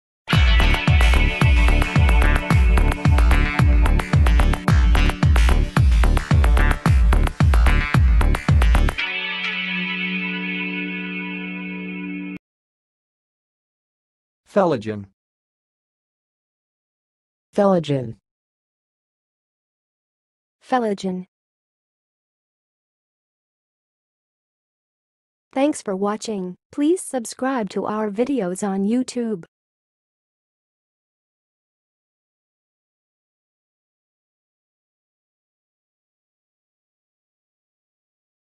How to say "phellogen"! (High Quality Voices)
Watch in this video how to say and pronounce "phellogen"!